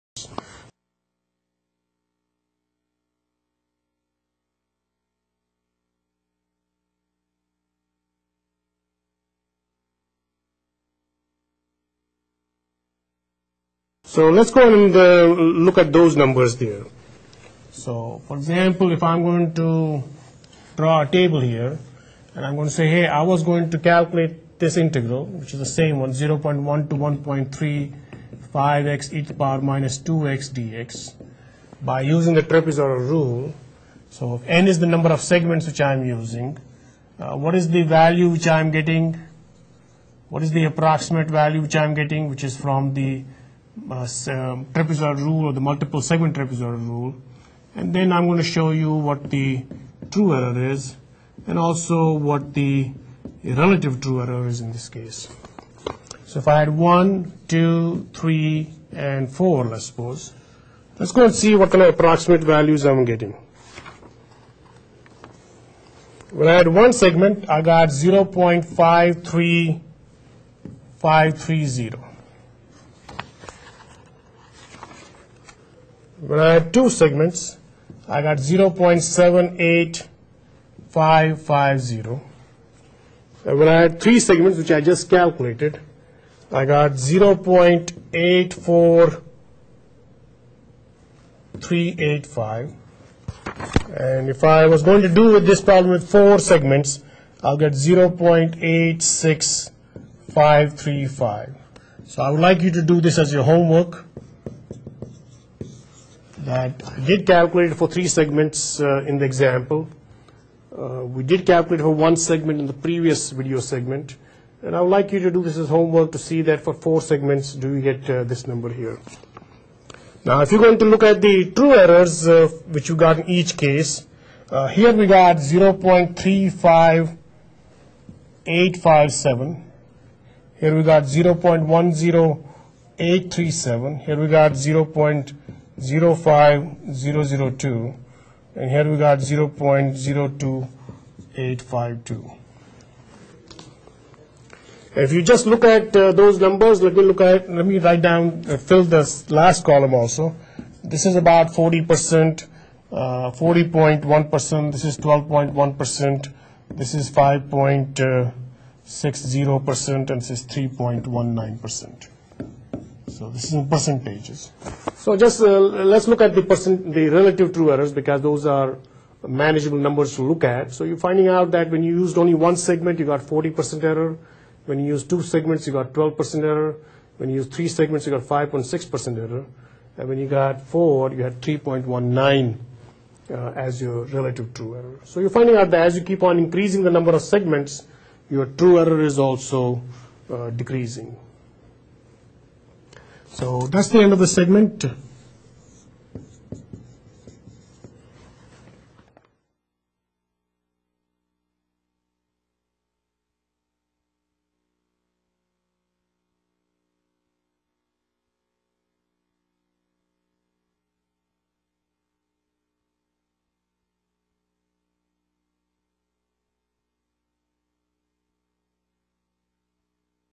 Multiple Segment Trapezoidal Rule: Example Part 2
Learn via an example the multiple segment trapezoidal rule. For more videos and resources on this topic, please